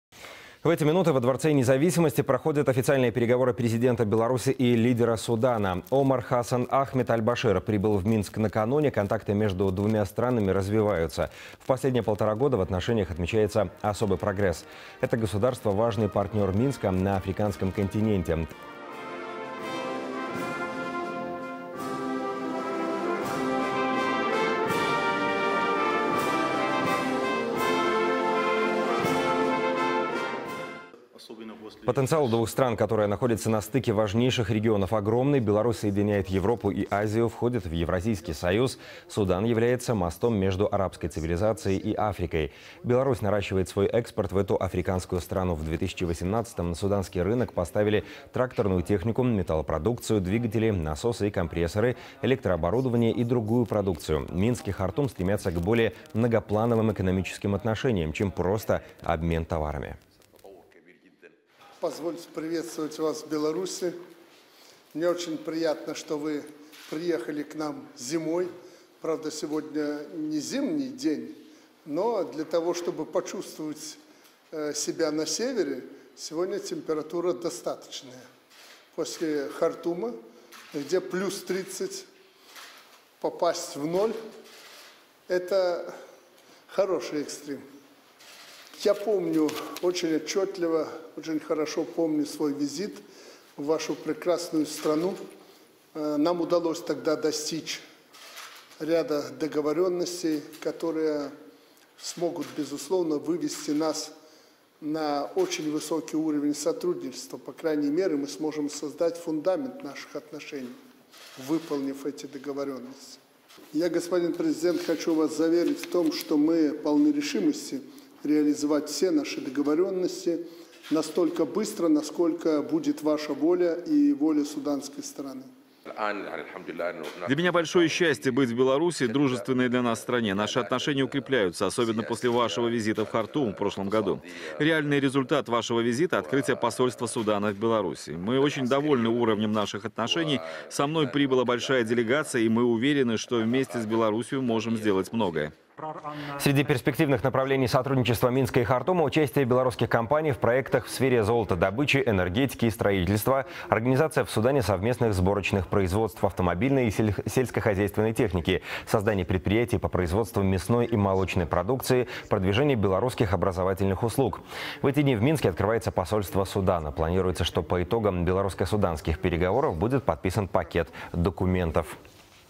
Во Дворце Независимости проходят официальные переговоры Президента Беларуси и лидера Судана
Омар Хасан Ахмед Аль-Башир прибыл в Минск накануне. Контакты между двумя странами развиваются. В последние полтора года в отношениях отмечается особый прогресс. Это государство – важный партнёр Минска на африканском континенте. Толчком для развития диалога стал визит нашего президента в Хартум зимой 2017 года.